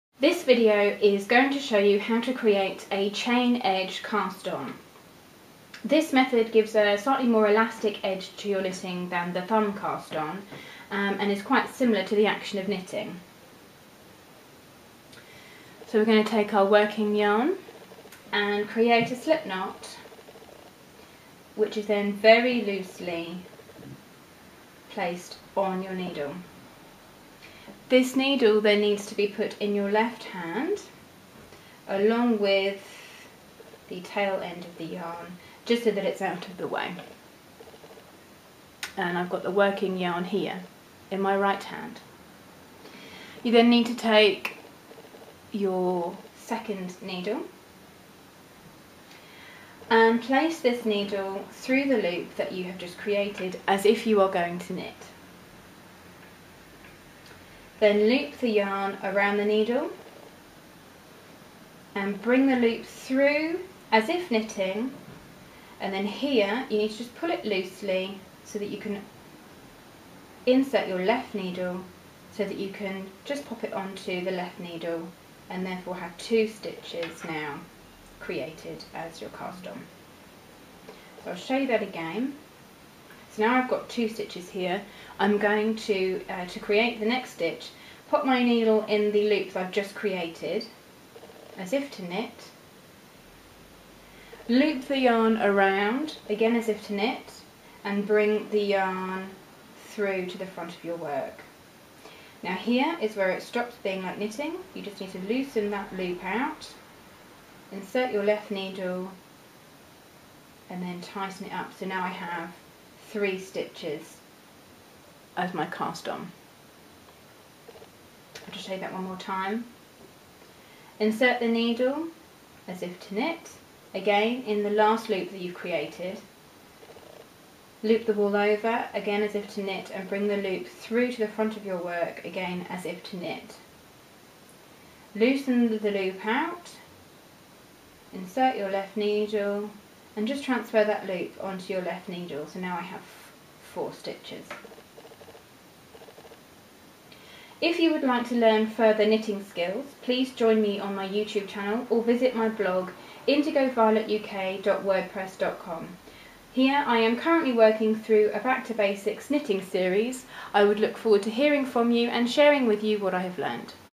Back To Basics Knitting: Chain Edge Cast On
How to cast on stitches using the chain edge cast on method.